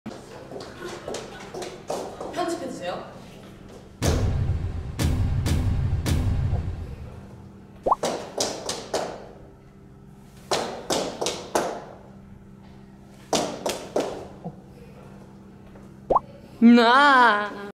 Heejin tries to make a scary sequence
But she is cute anyways

Source:
(3:09)